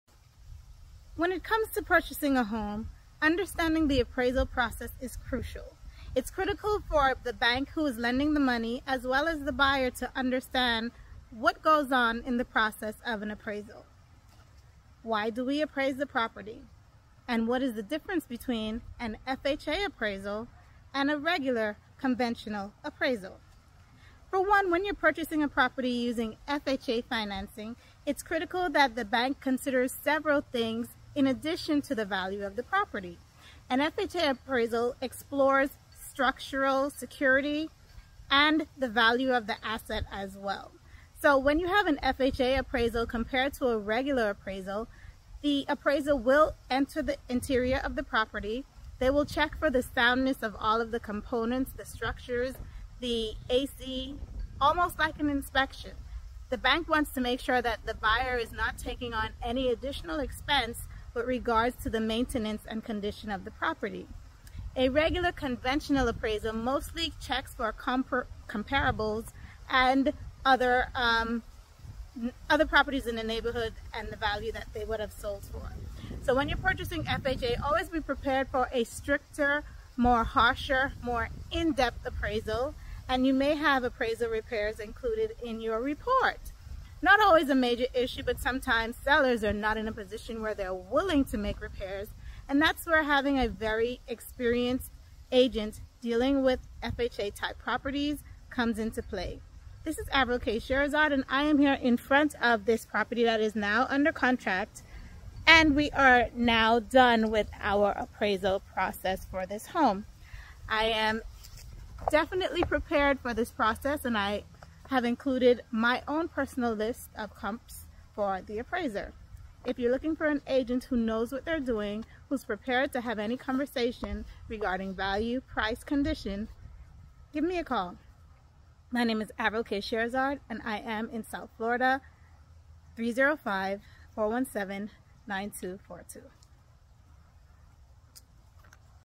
🔍 The differences between FHA and Conventional home appraisals! 🏠 Watch Avril K Cherasard now 📝💼
Hey there, house hunters! 🏠 Curious about what home appraisals are all about? 🤔

Our latest video breaks down the differences between FHA and Conventional appraisals so you can be a pro in the home buying game!

From safety checks to market values, we've got you covered. 📝

💼 Don't miss out! Contact Avril K Cherasard Miami Realtor now to level up your real estate knowledge! 🎥🏡📈